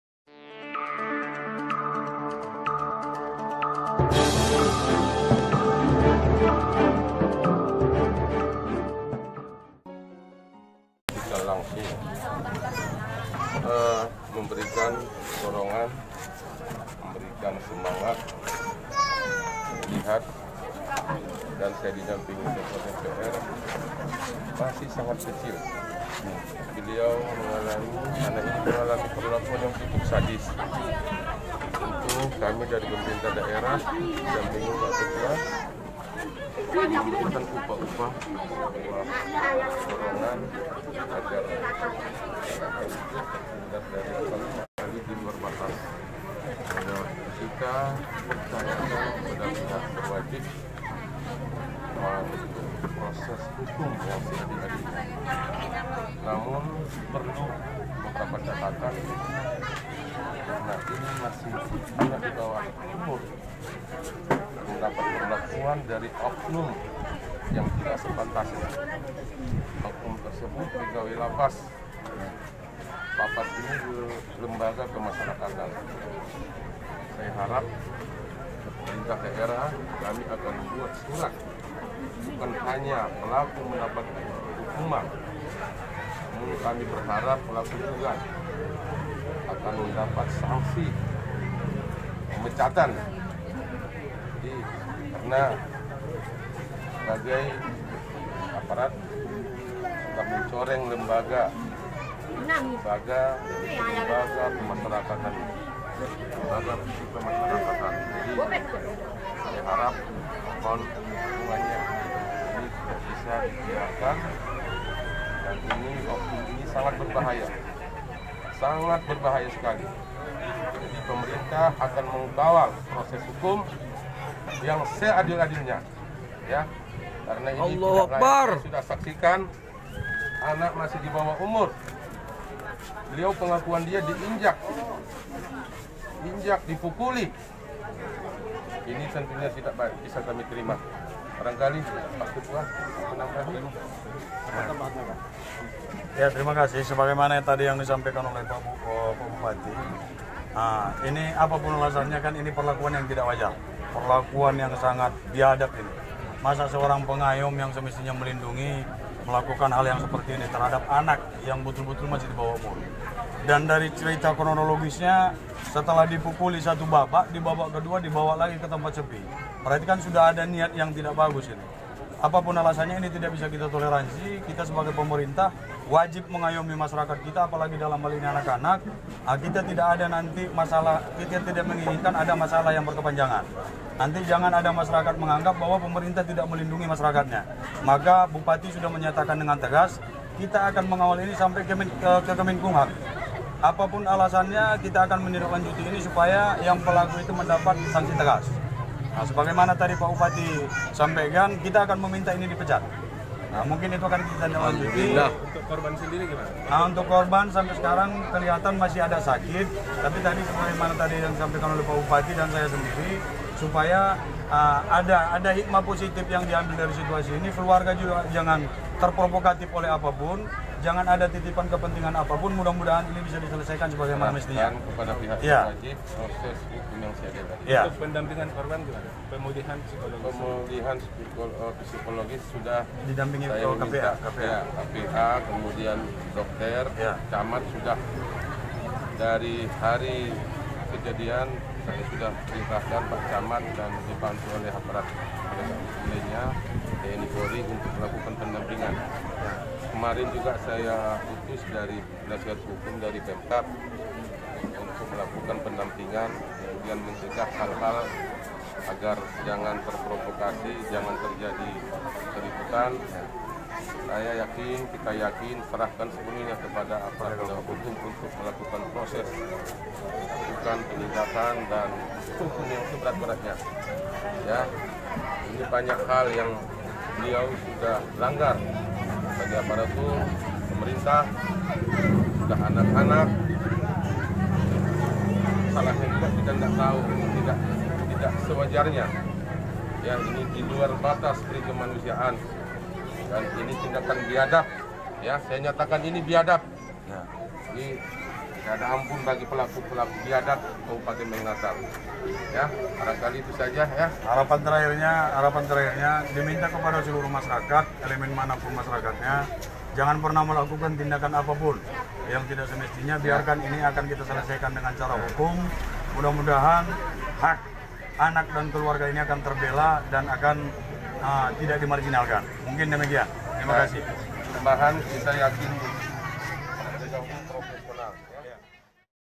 Bupati Madina & Ketua Dprd Kunjungi Warga Natal.
Bupati Beserta ketua DPRD Mandailing Natal Berharap Agar Pelaku di Tindak Tegas dan di Pecat.